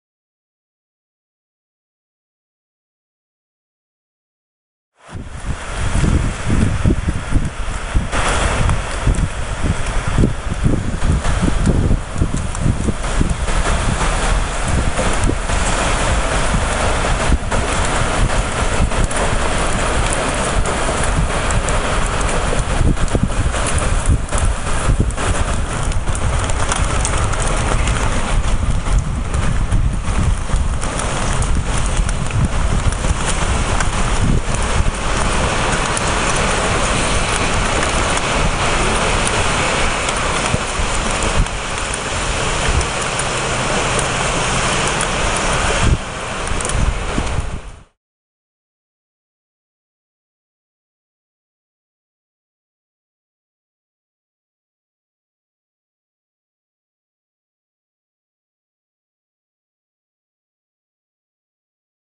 Rain Event Tallebudgera Creek Flooding below Gold Coast Hwy
Wild Weather Event Tallebudgera Creek Flooding, Water coming close to going over Gold Coast Highway Which will cut of Palm Beach